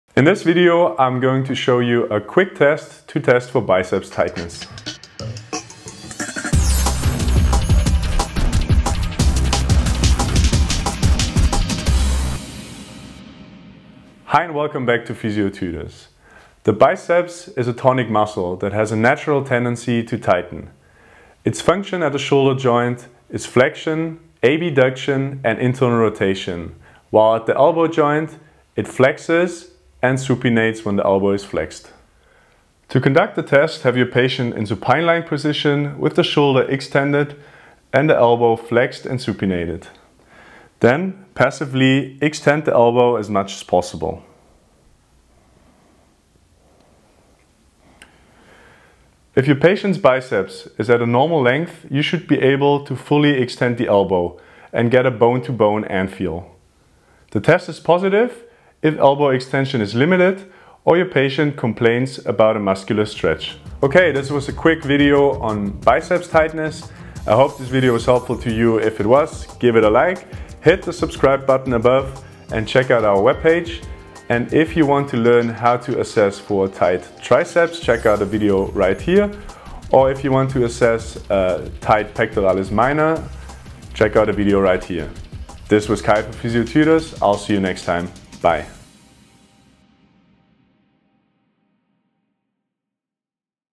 Biceps Brachii Muscle Tightness Test
The content is intended as educational content for health care professionals and students. If you are a patient, seek care of a health care professional. Here is how to test for biceps brachii muscle tightness

Your PhysioTutors